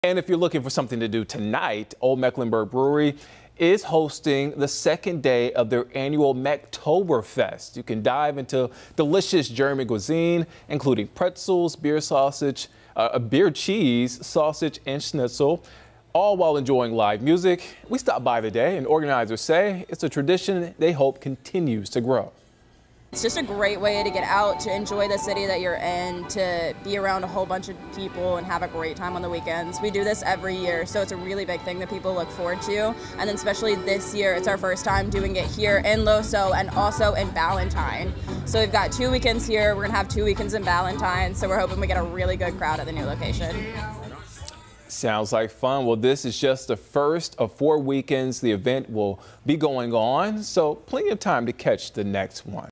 OMB kicks off annual Mecktoberfest in Charlotte
Olde Mecklenburg Brewery is kicking off its annual Mecktoberfest. You can drive into delicious German cuisine including pretzels, beer cheese, sausage and schnitzel, all while enjoying live music.

Organizers say it's a tradition they hope continues to grow.